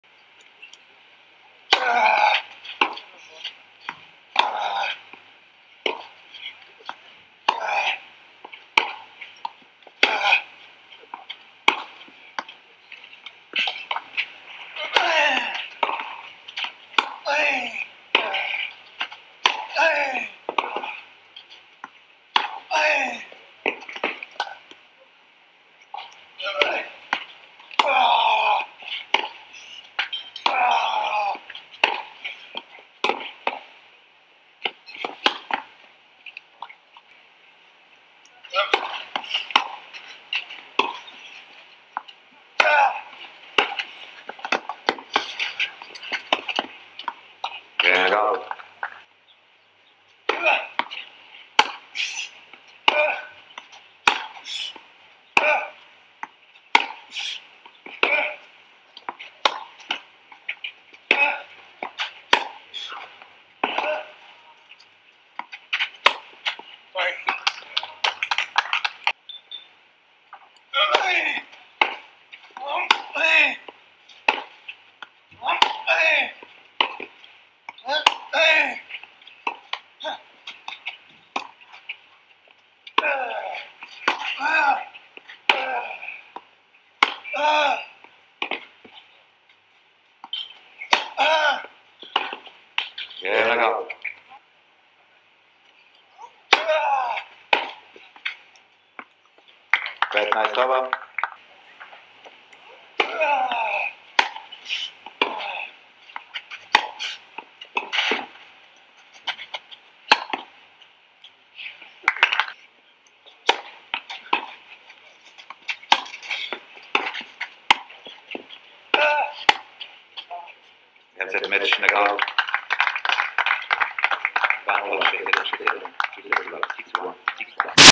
Sumit Nagal vs Matija Pecotic - ATP Banja Luka Challenger Round 3 Highlights
Sumit won 6-1 6-1 against the left handed Croatian WC